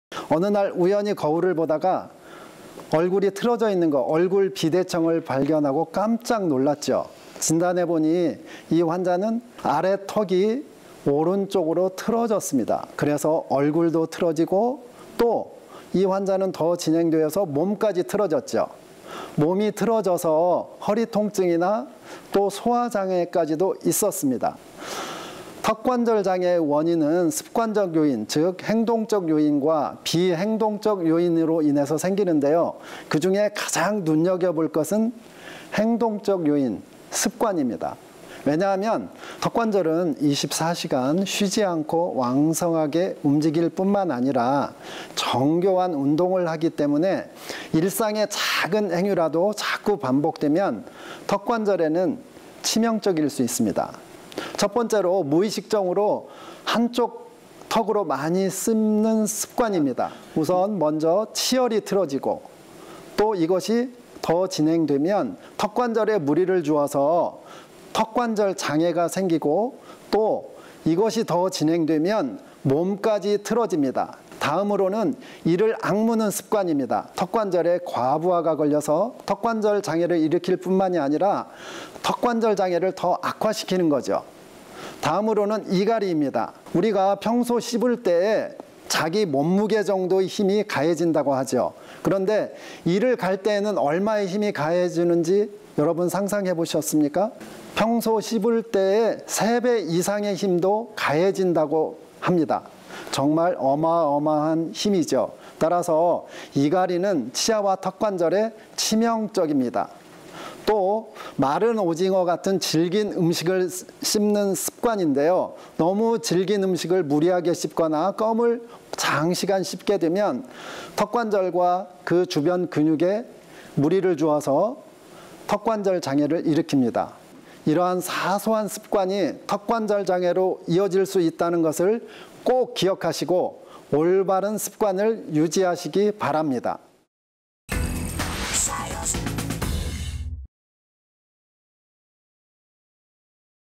껌을 장시간 자주 씹으면 턱관절장애를 일으킨다? / YTN 사이언스
몸의 균형까지 망칠 수 있는 턱관절장애.평소에 껌을 장시간 오래 씹거나 오징어와 같은 질긴 음식을 씹을 때 턱관절장애가 나타나며특히 수면 중 이갈이는 턱관절에 치명적이라고 하는데.. . 턱관절장애의 원인이 되는 생활 속 습관에 대해 알아본다.